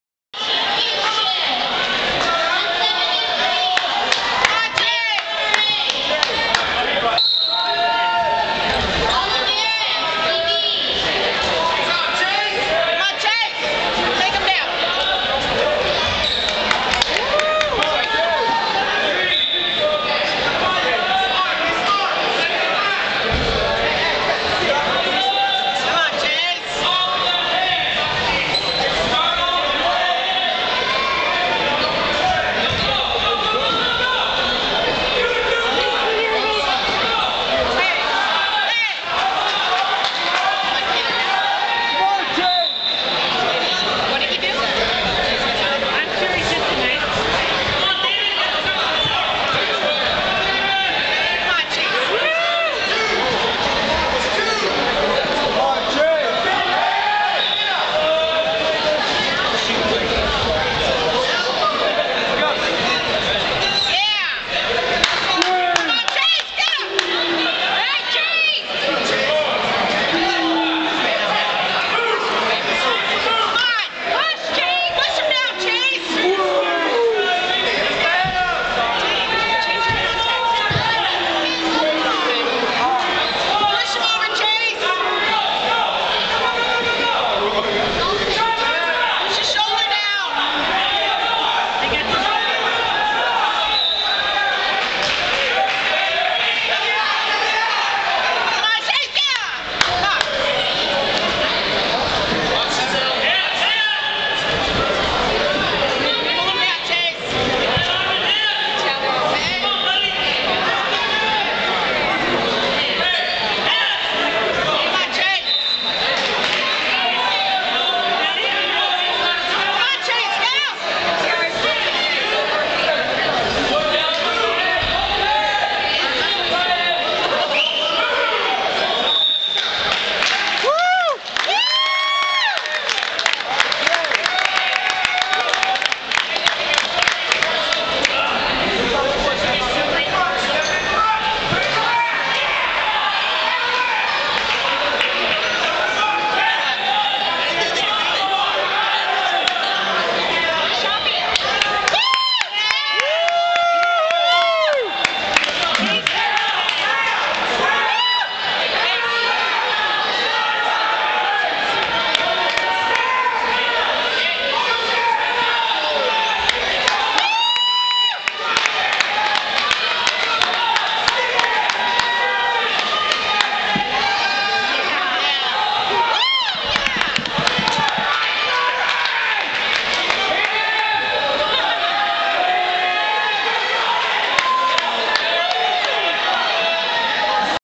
CHASE**ROUND 3** MBL FINALS- WRESTLING FOR 1ST PLACE 2/13/09
CHASE IS WEARING THE WHITE SINGLET ONLY WORN IN VARSITY CHAMPIONSHIP MATCHES. NEXT WEEK HE GOES TO CCS, IF HE WINS HE'LL CONTINUE ON TO CIF.  GO CONDORS!!!